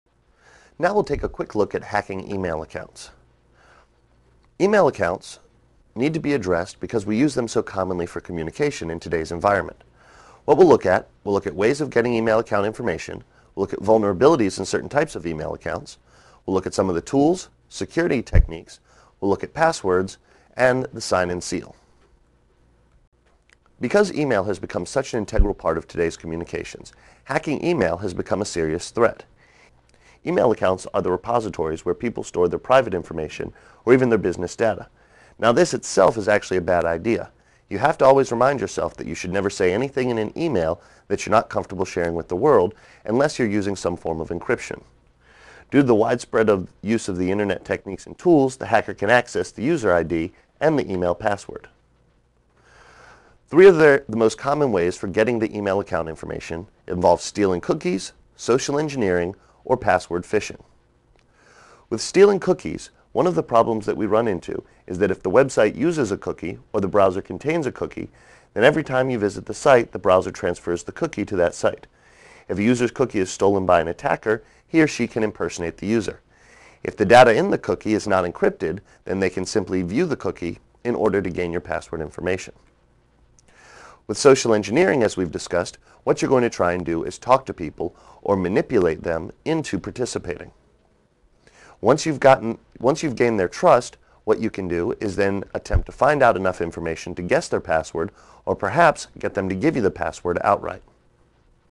CEH v6 Module 13: Hacking E-mail Accounts
Hacking email accounts demo from QuickCert's EC-Council CEH training course. QuickCert's Certified Ethical Hacker or CEH is a vendor-neutral certification that explores the existence and the use of hacking tools and security systems across 22 topical domains.